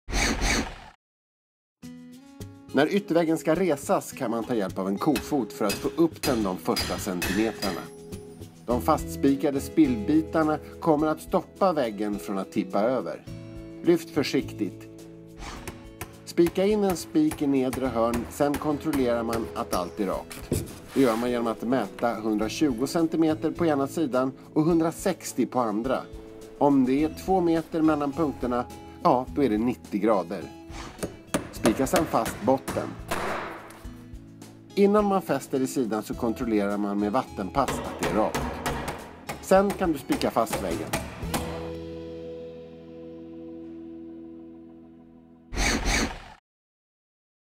FixarTV | Bygga Yttervägg (del 3) | Resa Yttervägg
När du ska resa din yttervägg måste du vara försiktig. Det är tungt, så ta gärna hjälp av någon i det här momentet, och se till att det är rakt när du fäster den.

Det här behöver du för att resa yttervägg:

● Yttervägg
● Kofot
● Tre stöd/träklossar
● Hammare
● Spik
● Tumstock
● Vattenpass

Så här gör du för att resa din yttervägg:

Steg 1. Om du inte redan gjort det kan du spika fast tre stöd i botten på reglarna. Dessa kommer att stoppa ytterväggen från att tippa över och välta.

Steg 2. Ta tag på var sin sida av ytterväggens ovansida.

Steg 3. Stoppa in en kofot för att få hjälp med att lyfta de första centimetrarna.

Steg 4. Res försiktigt ytterväggen så att den står upp.

Steg 5. Fäst med en spik i hörnet innan du kollar om det är rakt.

Steg 6. För att få en rak vinkel, ska du mäta nere vid golvet. 120 cm på ena sidan och 160 cm på den andra sidan. När du sedan lägger

Steg 7. tumstocken mellan dessa punkter skall det vara exakt 2 meter. Så har du fått en 90-gradig vinkel.

Steg 8. Dra ett sträck på golvet längs med den resa ytterväggen så att du vet var väggen skall så.

Steg 9. Slå i den första spiken till hälften, lägg till ett vattenpass så att du hela tiden kan kontrollera att det är rakt när du slår i den helt.

Steg 10. Nu kan du fortsätta med resten av spikarna så att väggen sitter ordentligt.